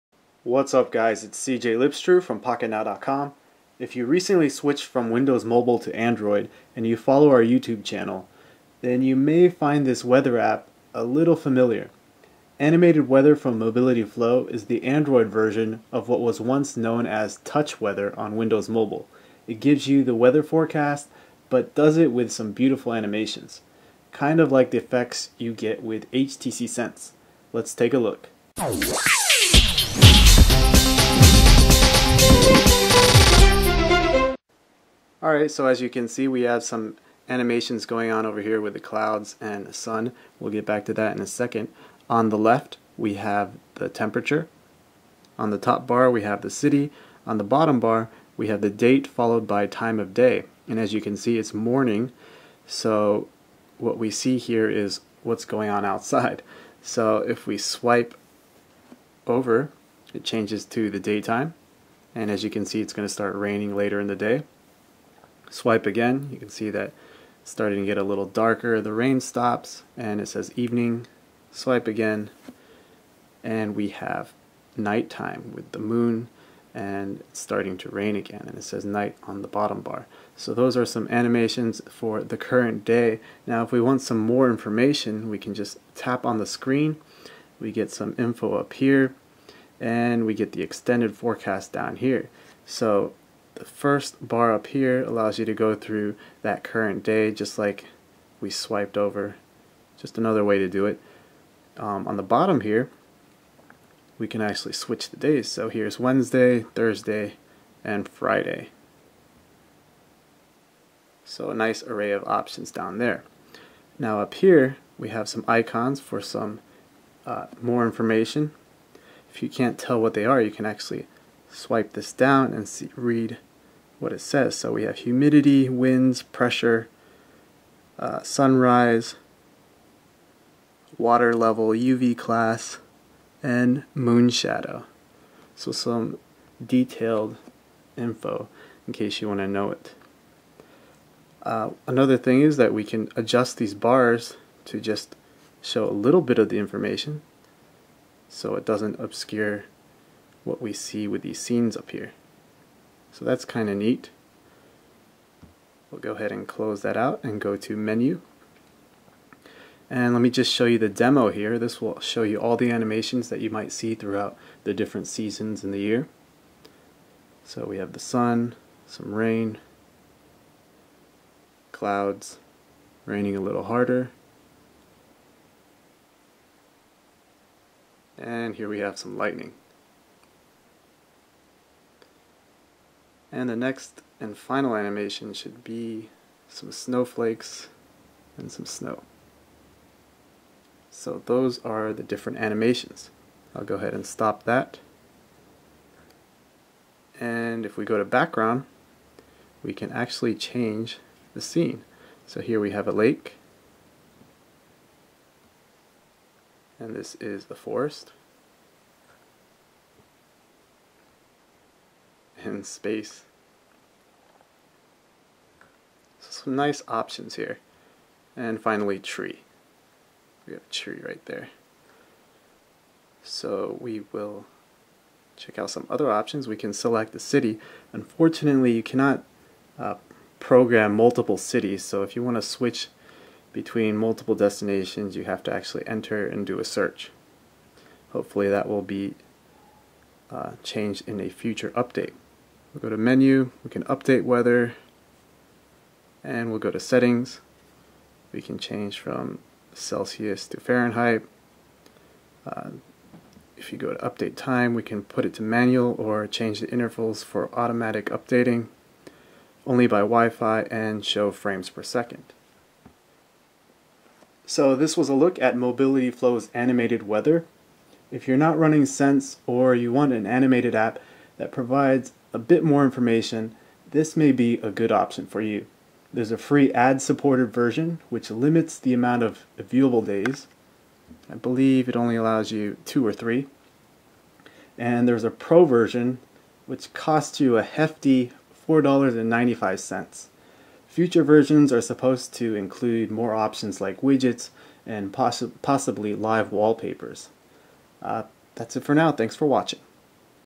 MobilityFlow Animated Weather App for Android | Pocketnow
Read In this video, we show you MobilityFlow's latest app, called Animated Weather. It's a port of their Windows Mobile version over to Android and brings some fancy animated effects.

Two versions: Free, ad-supported and Pro for $4.95 (will include widgets and other features in the future)